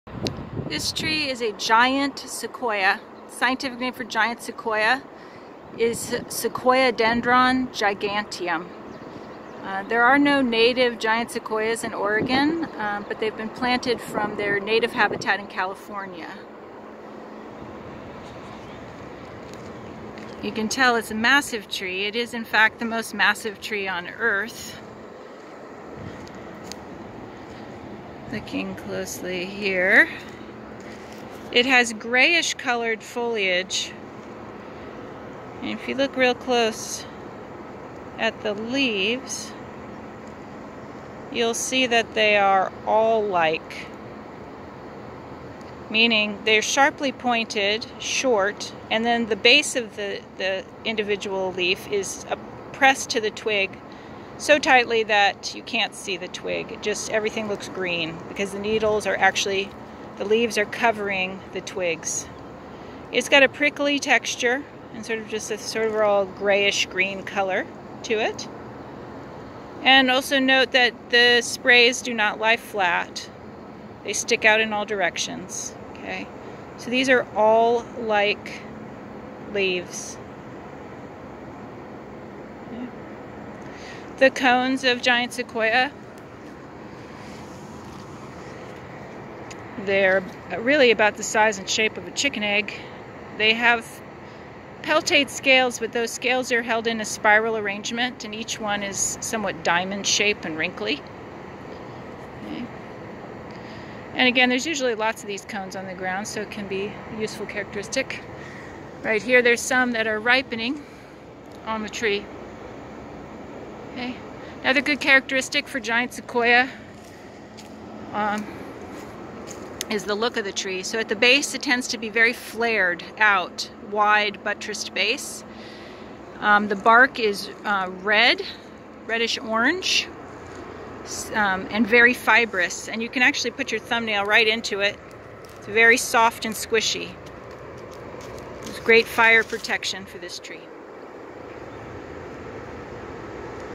Sequoiadendron giganteum
Identifying giant sequoia (Sequoiadendron giganteum). Recorded in Jensen Arboretum at Oregon State University.